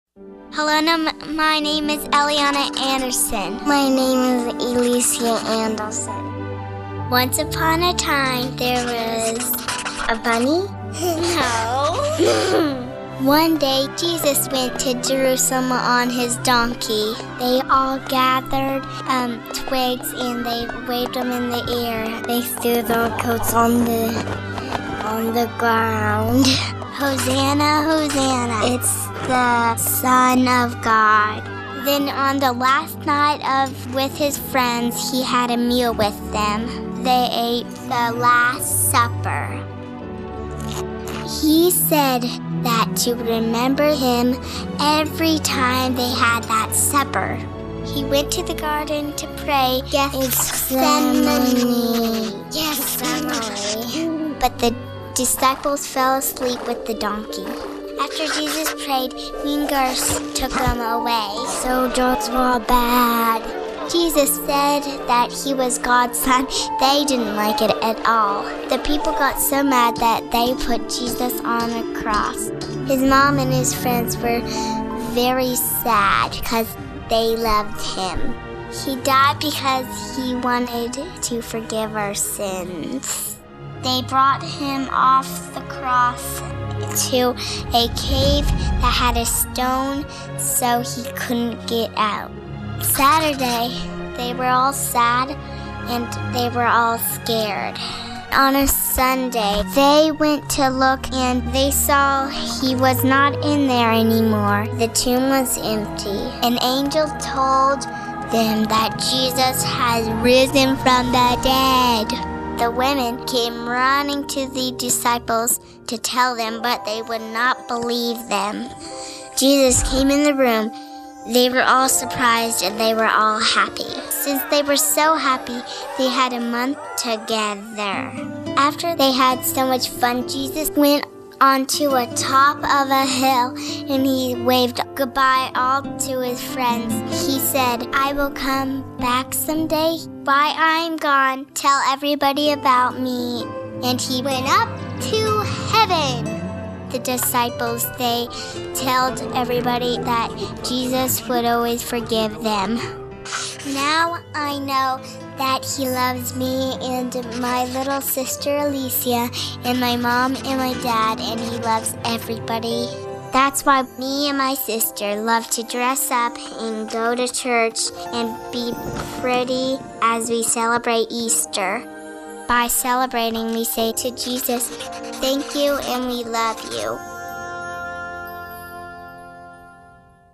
The Easter Story told by Haven & Miriam
Haven and Miriam share the Easter story with cartoon drawings